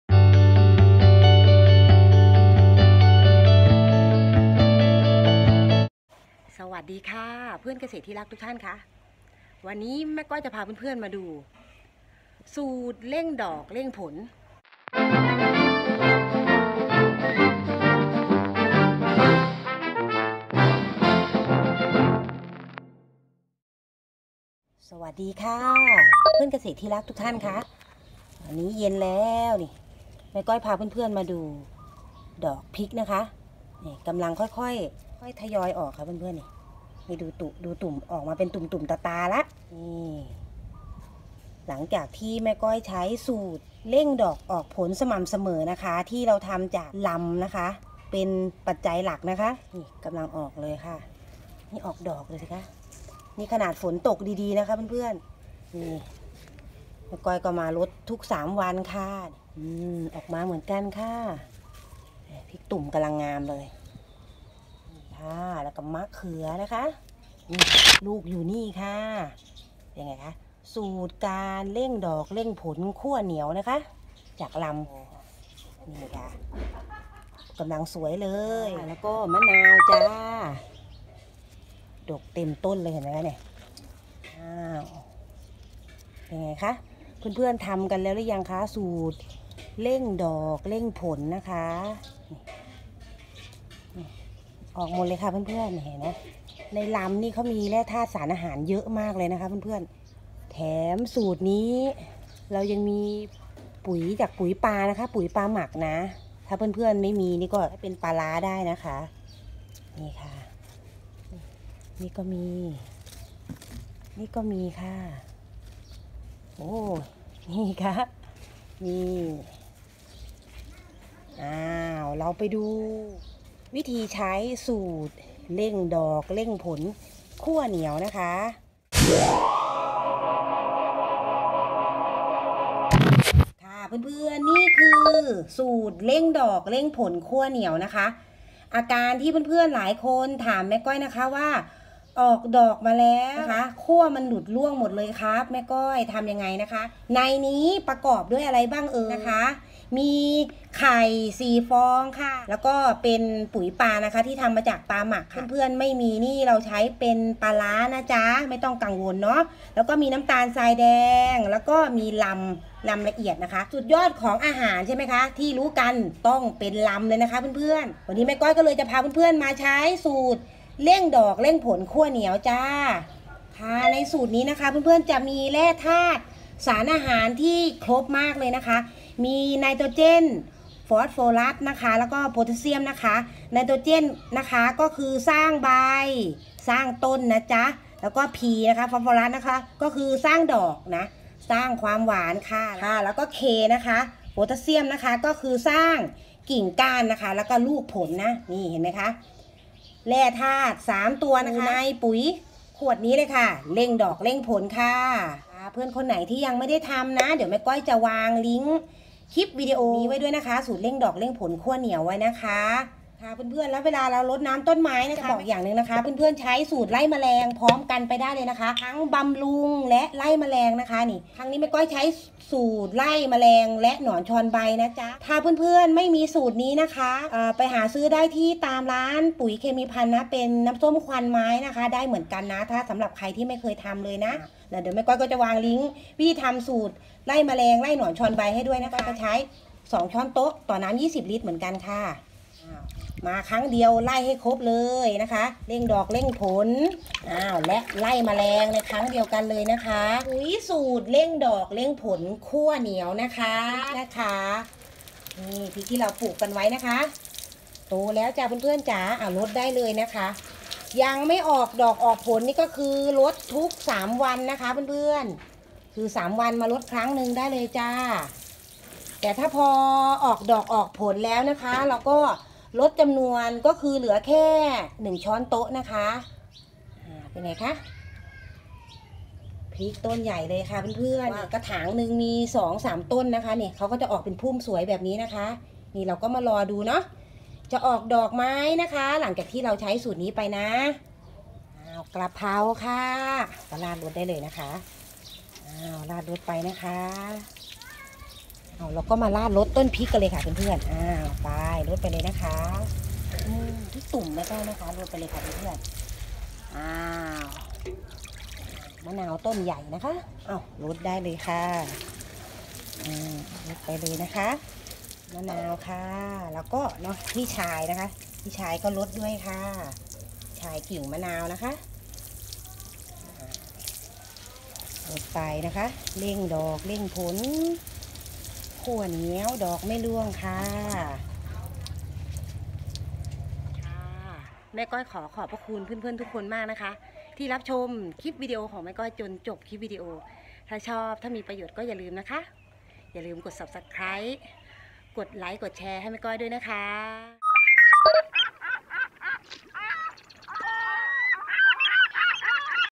แจกสูตร!!เร่งดอก เร่งผล ขั้วเหนียวดกจนกิ่งแทบหัก เก็บกินตลอดปีแม่ก้อยพาทำ
สวัสดีค่ะเพื่อนเกษตรที่น่ารักทุกคน แม่ก้อยขอพา เพื่อนมาใช้สูตร เร่งดอก เร่งผล  ขั้วเหนียวดกจนกิ่งแทบหัก!!  เก็บกินได้ตลอดปี
ผลเป็นอย่างไร  มาดูกันสูตรนี้แก้ปัญหา พืชผักไม่โต ไม่ออกดอกไม่ออกผล ลองใช้สูตรนี้เลยคะ
แม่ก้อยขอขอบคุณ เพื่อนเกษตรที่น่ารักทุกคนที่ กดติดตามกดไลด์ กดแชร์ ให้แม่ก้อยเสมอมา  ❤️😊ด้วย